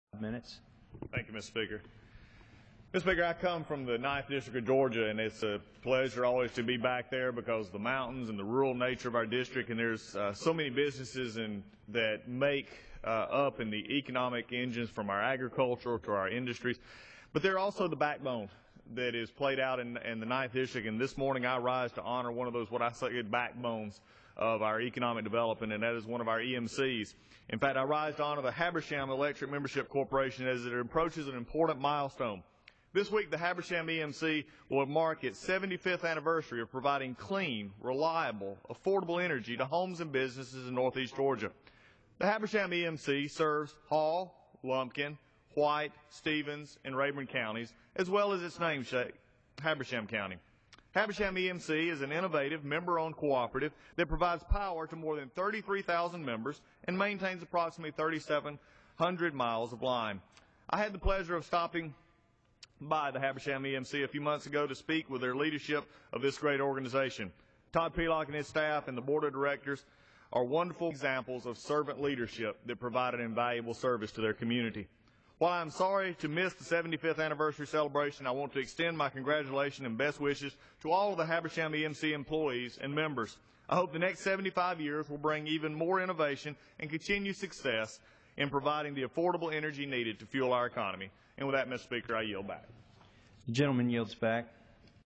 Rep. Collins Congratulations Habersham EMC on 75 Years
Congressman Doug Collins (GA-09) took to the House Floor earlier today to congratulate Habersham Electric Membership Corporation (EMC) on its 75th Anniversary. Thank you for your commitment to the people of Northeast Georgia!